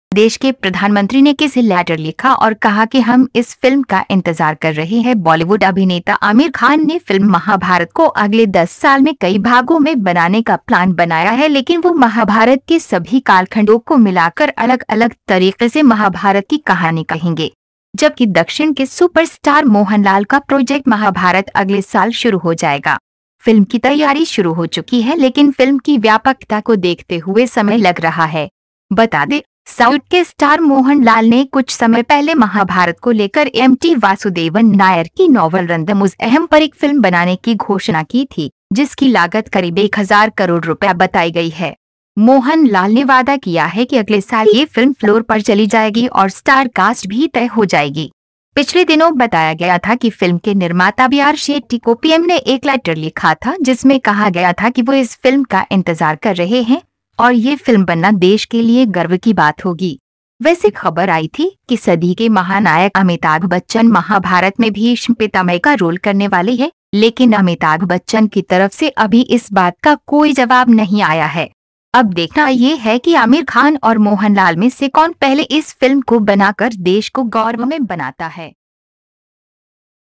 देश के प्रधानमंत्री ने किसे लैटर लिखा और कहा कि "हम इस फिल्म का इंतज़ार कर रहे है"
देश के प्रधानमंत्री ने किसे लैटर लिखा और कहा कि "हम इस फिल्म का इंतज़ार कर रहे है।" बॉलीवुड अभिनेता आमिर खान ने फिल्म 'महाभारत' को अगले दस साल में कई भागो में बनाने का प्लान बनाया है लेकिन वो महाभारत के सभी कालखंडो को मिलाकर अलग अलग तरीके से महाभारत की कहानी कहेंगे।

जबकि दक्षिण के सुपरस्टार मोहनलाल का प्रोजेक्ट महाभारत अगले साल शुरू हो जाएगा। फिल्म की तैयारी शुरू हो चुकी है लेकिन फिल्म की व्यापकता को देखते हुए समय लग रहा है। बता दे, साऊथ के स्टार मोहन लाल ने कुछ समय पहले महाभारत को लेकर एम् टी वासुदेवन नायर की नॉवल 'रंदमुज़हम' पर एक फिल्म बनाने की घोषणा की थी, जिसकी लागत करीब 1000 करोड़ रूपये बताई गई है।

मोहन लाल ने वादा किया है कि अगले साल ये फिल्म फ्लोर पर चली जाएगी और स्टारकास्ट भी तय हो जाएगी। पिछले दिनों बताया गया था कि फिल्म के निर्माता बी आर शेट्टी को पीएम ने एक लैटर लिखा था जिसमे कहा गया था कि वो इस फिल्म का इंतज़ार कर रहे है और ये फिल्म बनना देश के लिए गर्व की बात होगी।

वैसे खबर आई थी कि सदी के महानायक अमिताभ बच्चन महाभारत में भीष्म पितामय का रोल करने वाले है, लेकिन अमिताभ बच्चन की तरफ से अभी इस बात का कोई जवाब नहीं आया है। अब देखना ये है कि आमिर खान और मोहनलाल में से कौन पहले इस फिल्म को बनाकर देश को गौरवमय बनाता है।